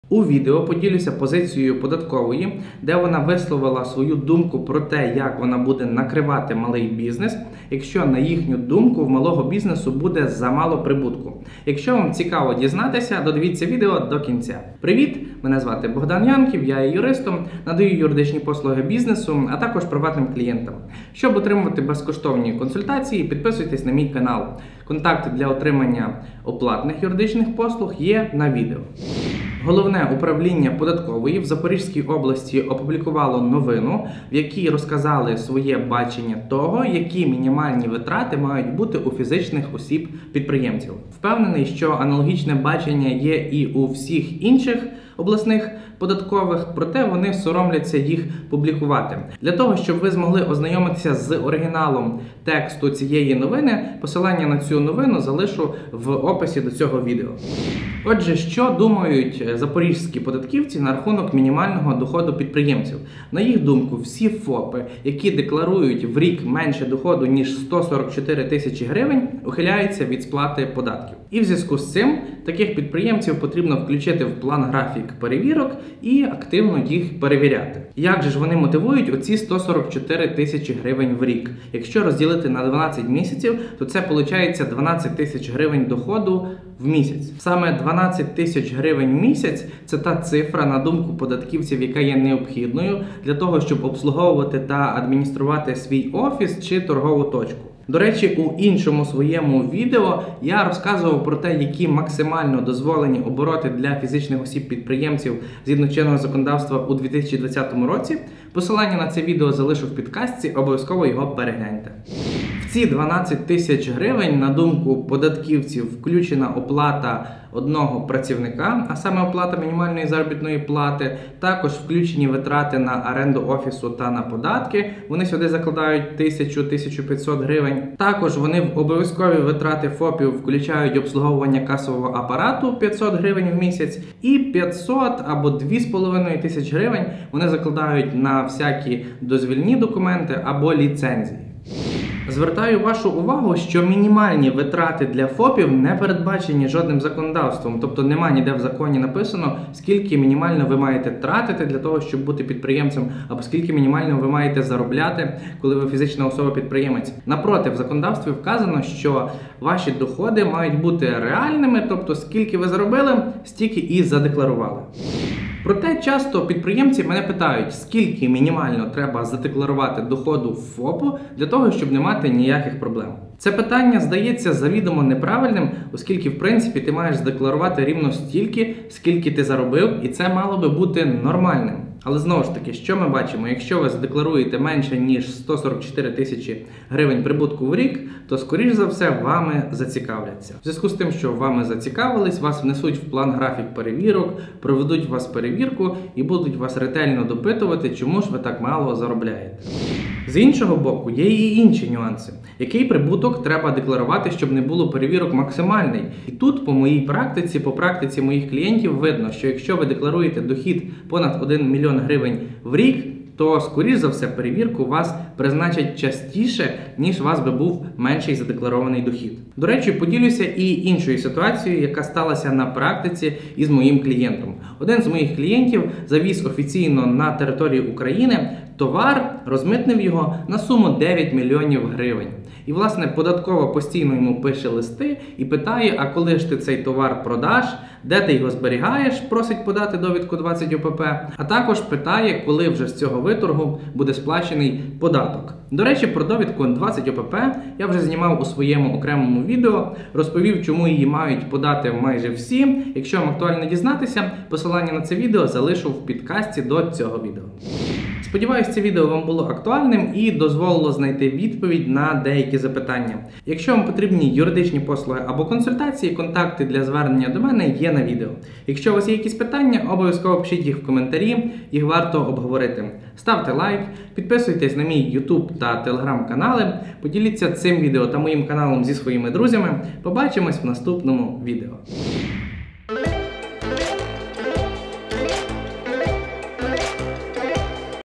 Які мінімальні витрати підприємця на думку податкової | Як податкова хоче накрити малий бізнес
Які мінімальні витрати підприємця на думку податкової | Як податкова хоче накрити малий бізнес - розповідає юрист Богдан Янків.